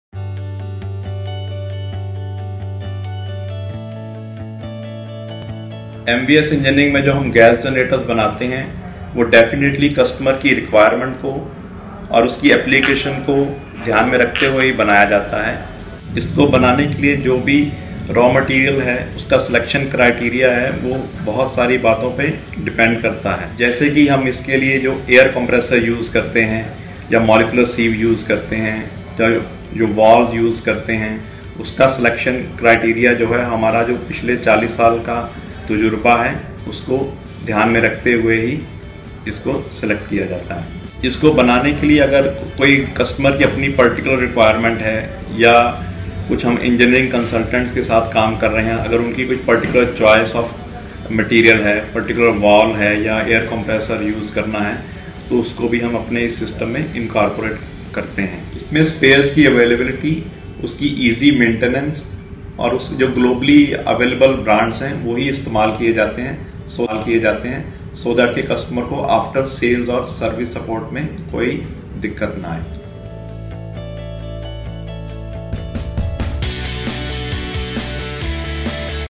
BEHIND THE SCENES - MVS - EPISODE 3 - SELECTION OF RAW MATERIAL
During the design phase, one of the major considerations our team takes is the careful selection of raw material and components. There are several factors that are accounted for, such as, vendor past performance, component longevity, future spares availability, among others.

Having 41+ years of experience gives us the ability to check back our records over past few decades as we go through the selection process and further guide our customers.

In case the customer has any specific preference or have an engineering consultant that has stipulated some specifications, then we consider those as well.